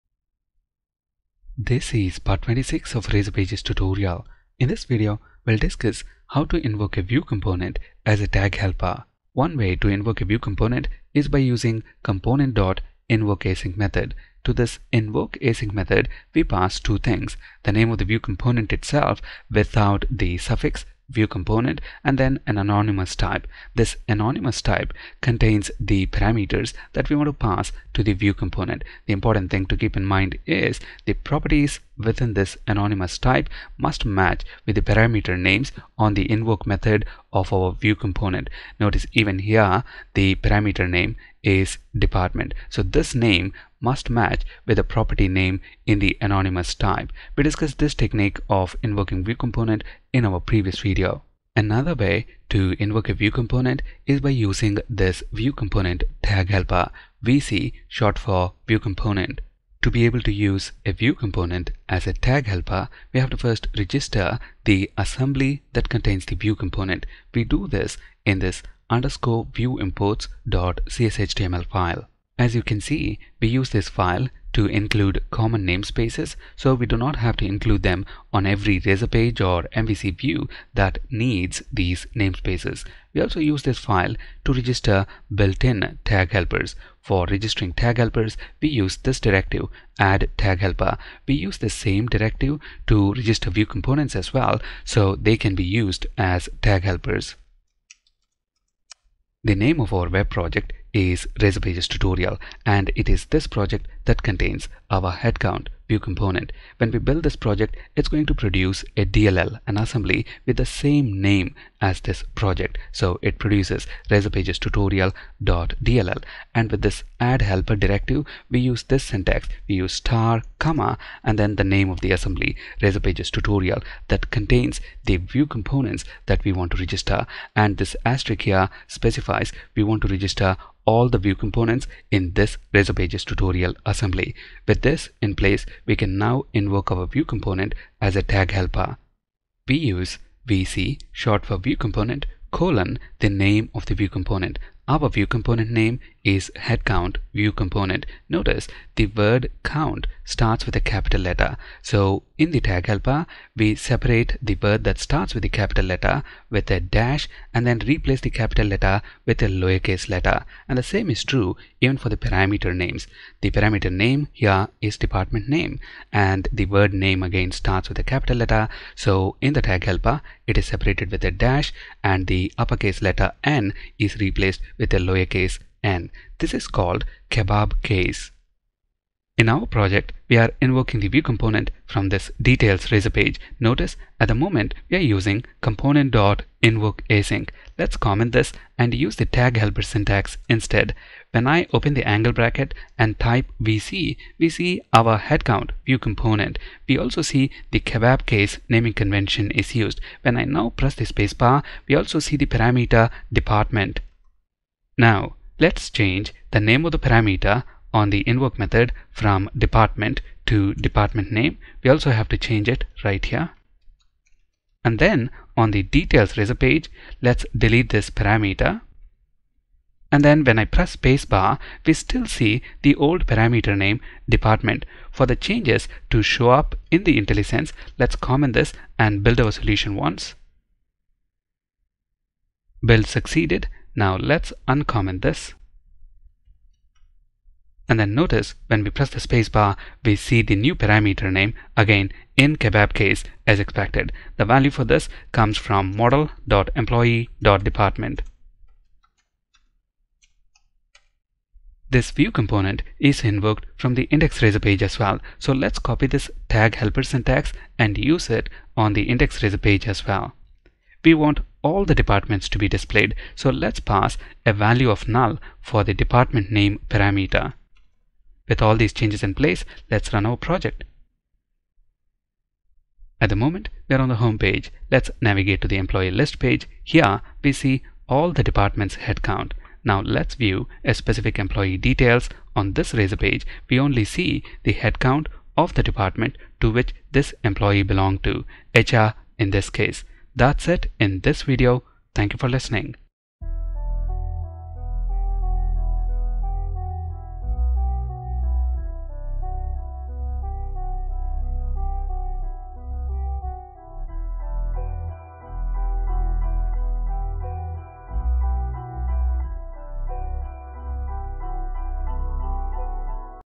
ASP NET Core view component tag helper
How to invoke a view component using View Component (vc) tag helper.